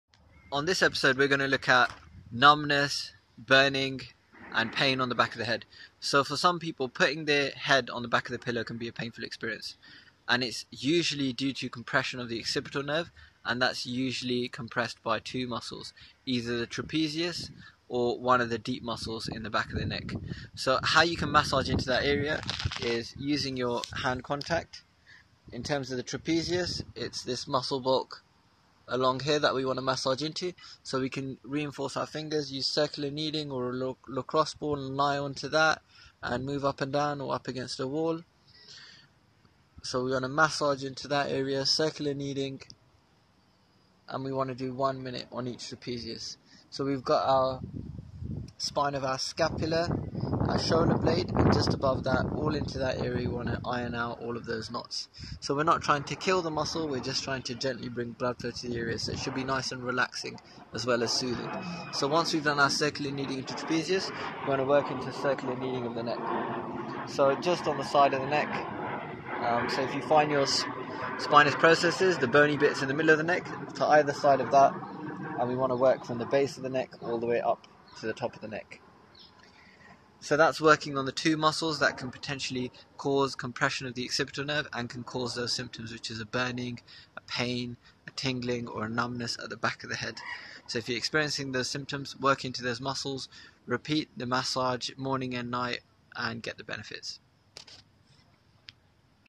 Burning On The Back Of The Scalp | Feat. Harbir Singh
Pain or burning on the back of the scalp is often due to impingement of a nerve in the neck. This video shows you how to relieve it.